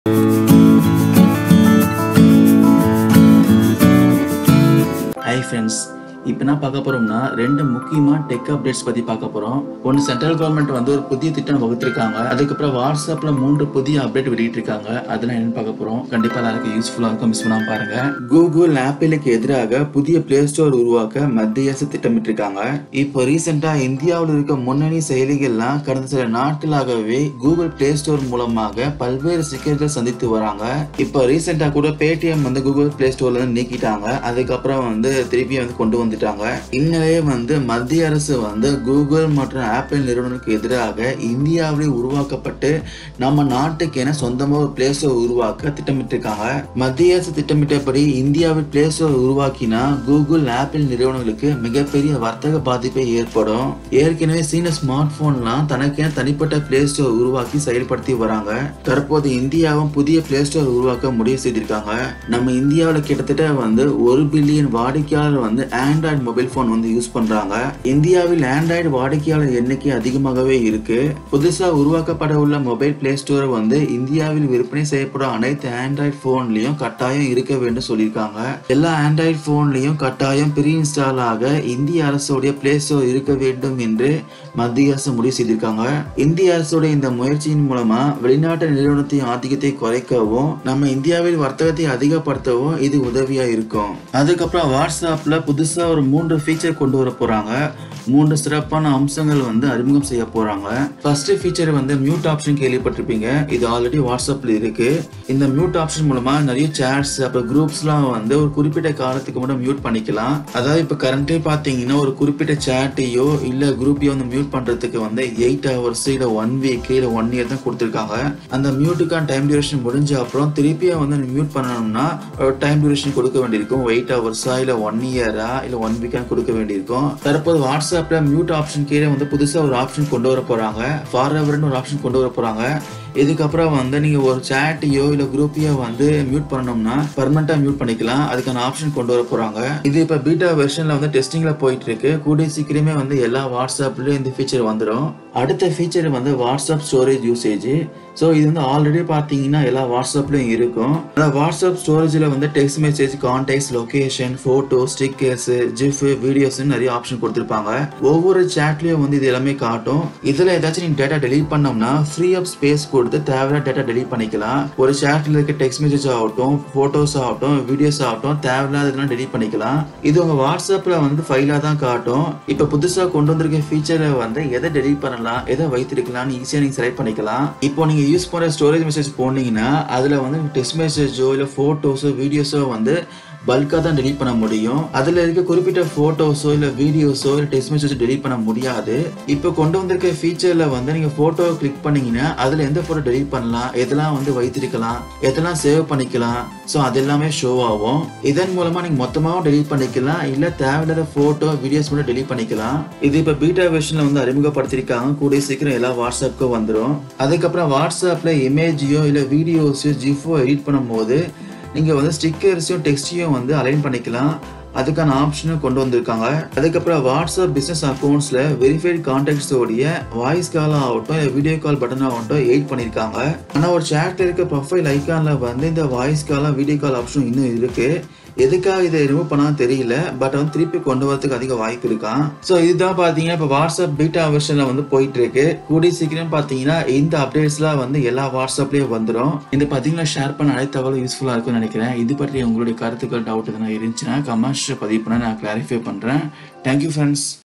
பிரதமர் மோடி மெகா திட்டம் நாடு முழுவதும் புதிய திட்டம்! Whatsappல் வருகிறது மூன்று புதிய அம்சம்
பிரதமர் மோடி மெகா திட்டம்! நாடு முழுவதும் புதிய திட்டம்! கூகிள், ஆப்பிள்-க்கு வருகிறது ஆப்பு !  Whatsappல் வருகிறது மூன்று புதிய அம்சம் இனி இந்த பிரச்சனை இருக்காது

Credits: Pexels, Pixabay

1. கூகிள், ஆப்பிள்-க்கு எதிராகப் புதிய ப்ளே ஸ்டோர்..? மத்திய அரசு திடீர் ஆலோசனை..!

இந்தியாவில் இருக்கும் முன்னணி செயலிகள் கடந்த சில நாட்களாகவே கூகிள் ப்ளே ஸ்டோரில் மூலம் பல்வேறு சிக்கல்களைச் சந்தித்து வருகிறது. இந்நிலையில் மத்திய அரசு கூகிள் மற்றும் ஆப்பிள் நிறுவனங்களுக்கு எதிராக இந்தியாவிலேயே உருவாக்கப்பட்டு நாம் நாட்டுக்கெனச் சொந்தமாக ப்ளே ஸ்டோர் உருவாக்கத் திட்டமிட்டு ஆலோசனை நடத்தி வருகிறது என அரசு தரப்புத் தகவல்கள் வெளியாகியுள்ளது.

மத்திய அரசு திட்டமிட்டபடி இந்தியாவில் ப்ளே ஸ்டோர் உருவாக்கினால் கூகிள், ஆப்பிள் நிறுவனங்களுக்கு மிகப்பெரிய வர்த்தகப் பாதிப்பை ஏற்படும்.

ஏற்கனவே சீன ஸ்மார்ட்போன்கள் தனக்கெனத் தனிப்பட ப்ளே ஸ்டோர் உருவாக்கி வரும் நிலையில் இந்தியாவில் இந்த முடிவு, இன்னும் பல நாடுகளைத் தனி ப்ளே ஸ்டோர் அமைக்க ஊக்குவிக்கும்.

2. வாட்ஸ் அப்பில் வருகிறது மூன்று புதிய அம்சம்: இனி இந்த பிரச்சனை இருக்காது!

வாட்ஸ்அப்பில் புதிய அம்சம் விரைவில் அறிமுகமாக உள்ளது. வாட்ஸ்அப்பில் வரவிருக்கும் புதிய அம்சங்கள் முதற்கட்டமாக பீட்டா பயனர்களுக்கு கிடைக்கும். அதன்பின்னரே அனைத்து பயனர்களுக்கும் கிடைக்கும் என தெரிவிக்கப்படுகிறது.

மூன்று புதிய அம்சங்கள்
வாட்ஸ்அப்பில் வரவிருக்கும் மூன்று புதிய அம்சங்கள் குறித்து பார்க்கையில், அது ஆல்வேஸ் மியூட், புது சேமிப்பு பயன்பாடு, ஆண்ட்ராய்டு பீட்டா பதிப்பில் சோதனை மேற்கொள்வதற்கான ஊடக வழிகாட்டுதல்கள் உட்பட பல அம்சங்கள் வெளியாகிறு